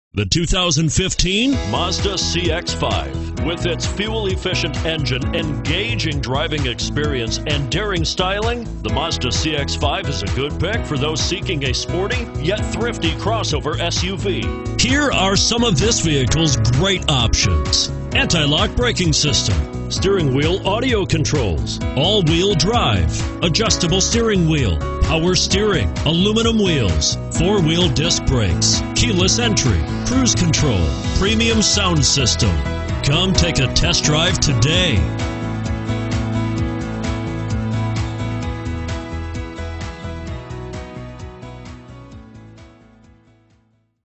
2015 MAZDA CX-5 Egg Harbor, NJ 5C15083
2015 MAZDA CX-5 Egg Harbor, NJ
Stock# 5C15083

Pick up this Crystal White Mica 2015 Mazda CX-5, available today. Featuring Automatic transmission, it has 0 miles. This could be the one you've been searching for.